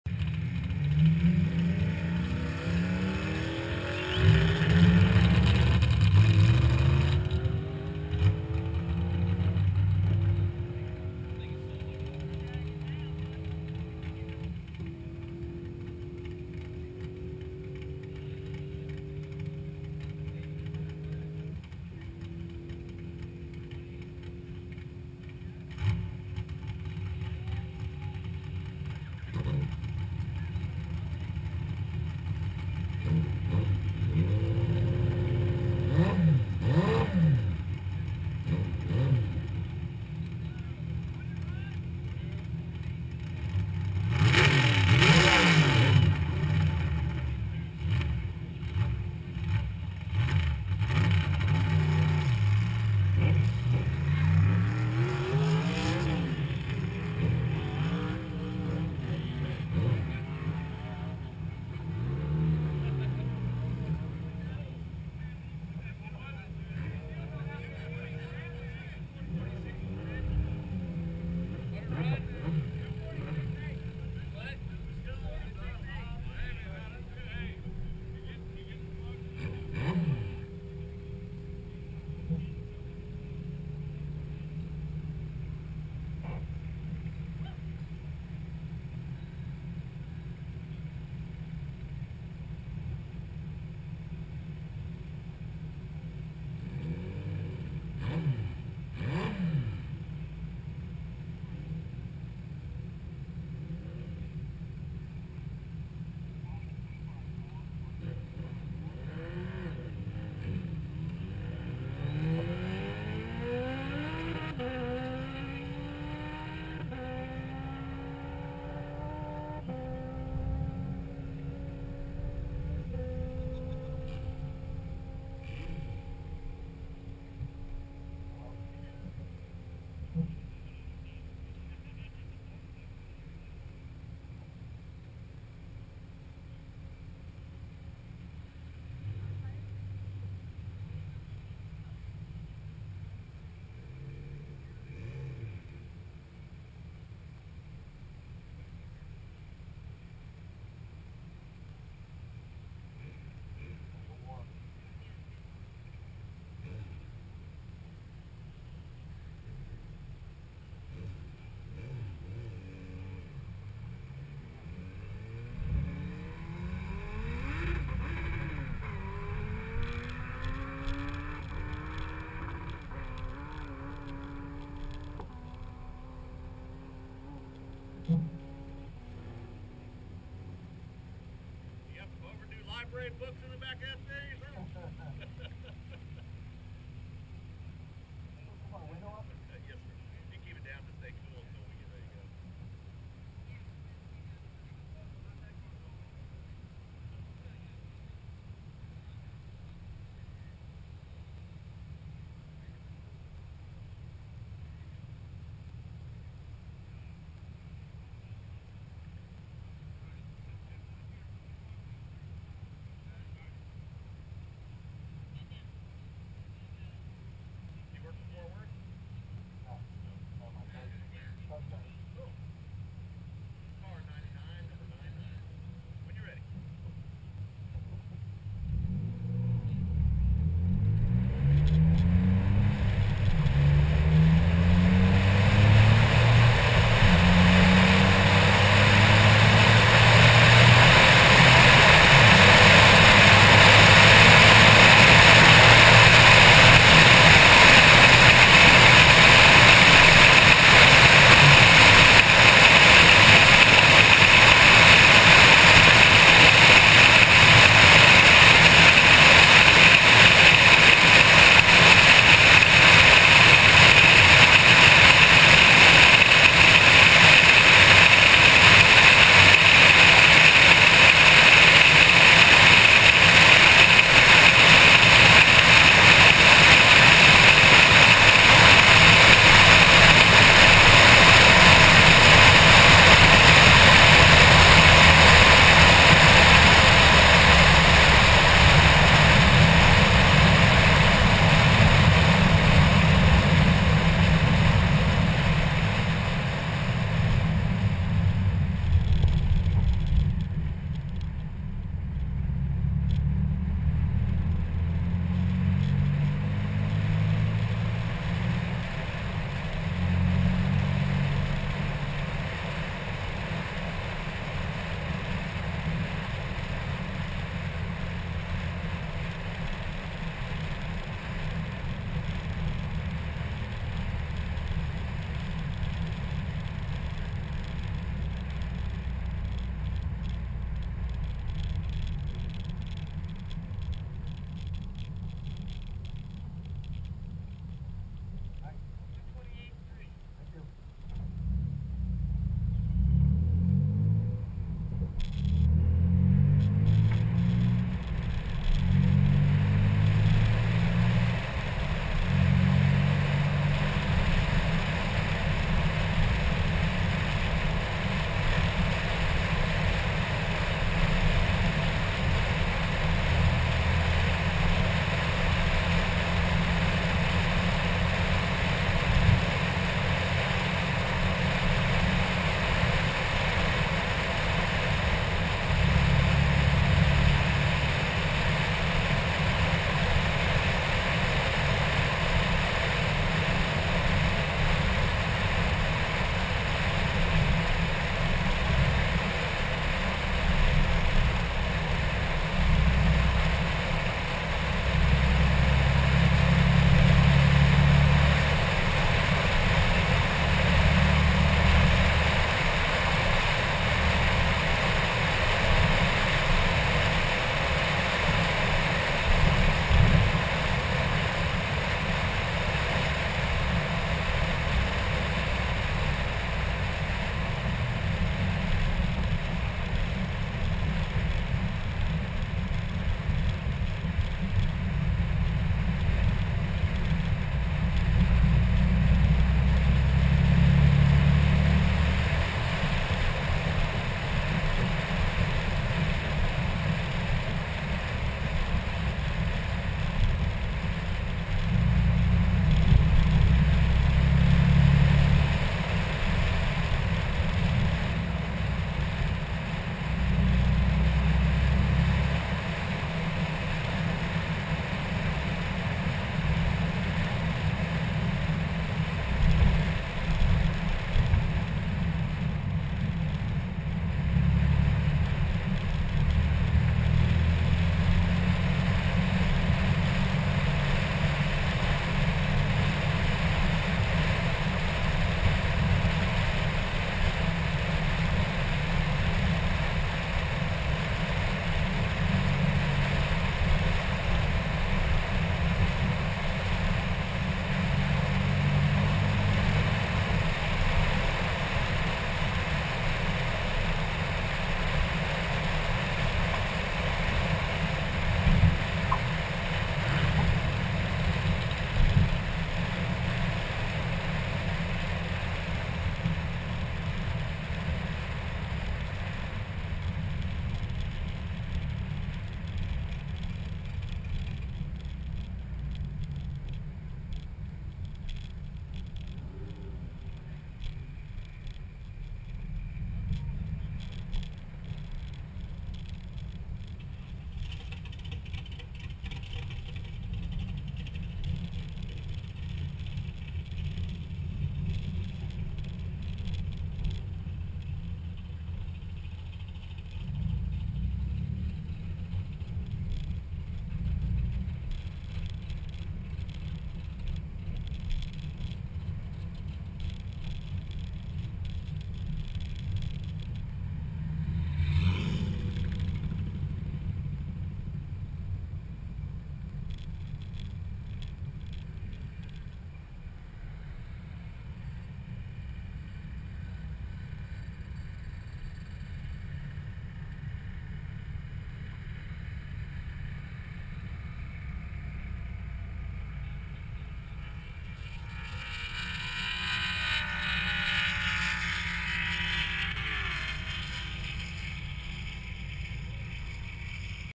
Fastest Maxima at the Texas Mile 3/24/12
Texas Mile 3/24/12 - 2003 Nissan Maxima - 131 mph
That's one sweet F Body too.